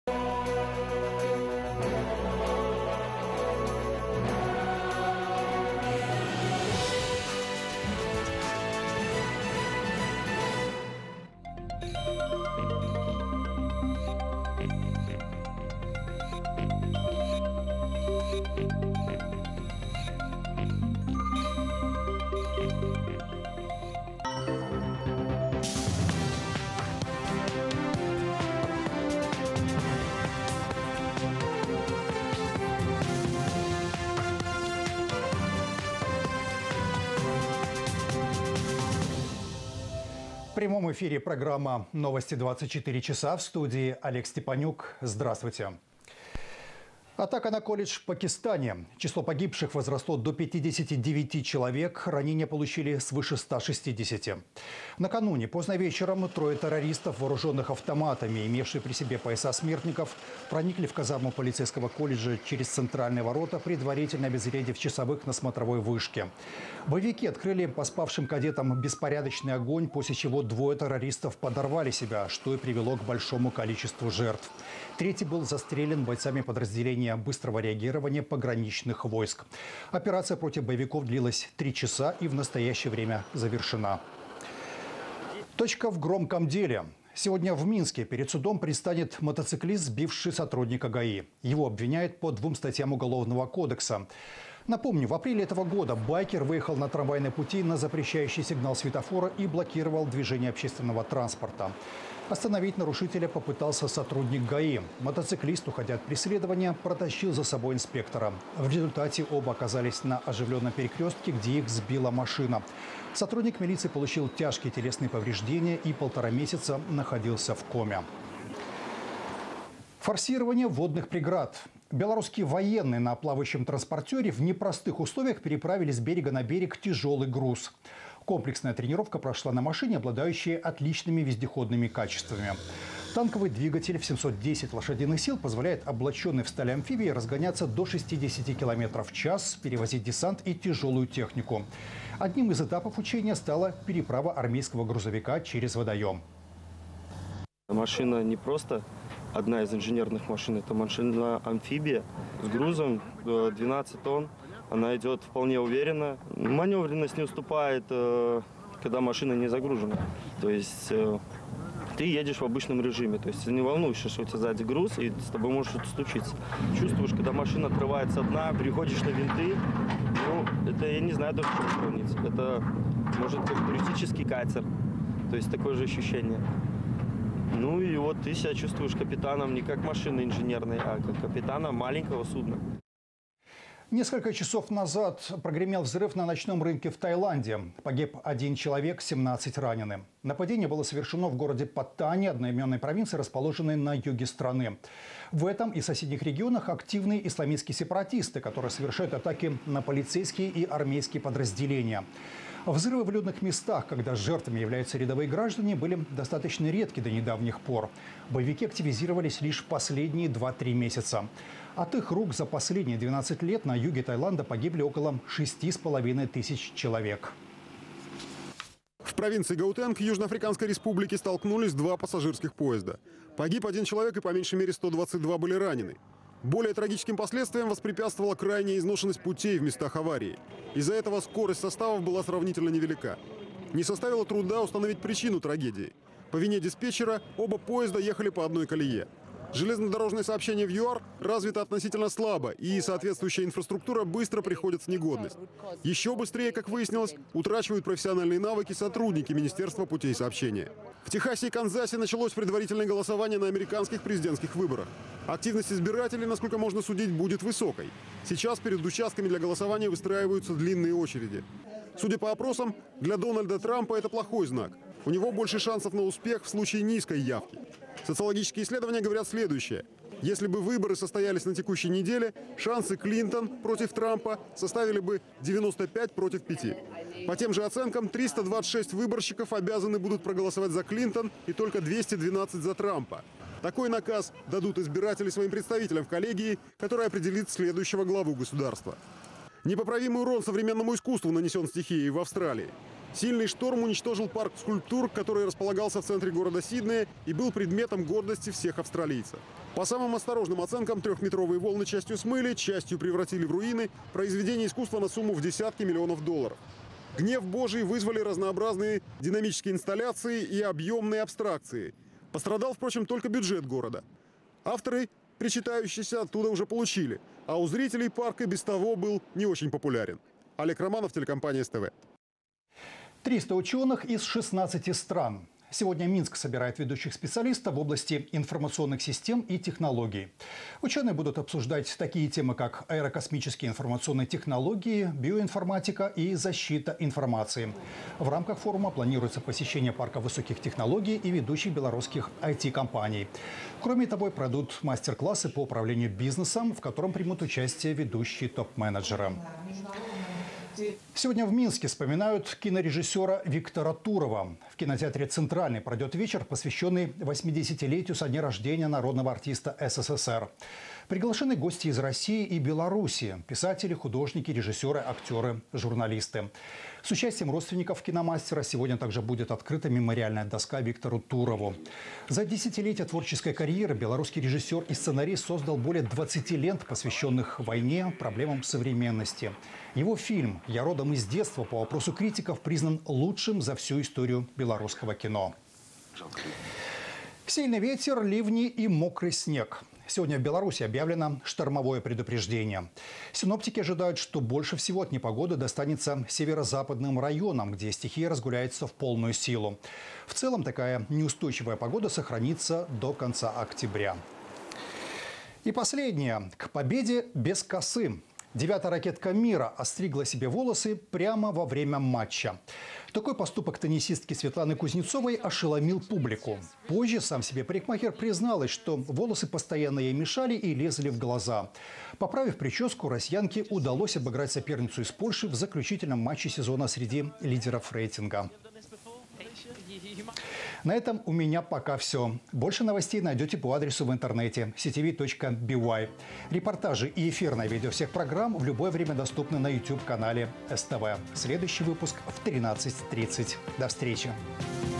Новости "24 часа" за 10.30 25.10.2016
Террористы атаковали полицейский колледж в Пакистане: погибли не менее 59 человек, ранены свыше 160
Взрыв на ночном рынке в Таиланде: погиб один человек, 17 ранены
Два пассажирских поезда столкнулись в ЮАР: погиб 1 человек, ранены не менее 122
Сильный ветер, ливни и мокрый снег: в Беларуси объявлено штормовое предупреждение
Российская теннисистка Светлана Кузнецова остригла себе косу прямо во время матча и в итоге победила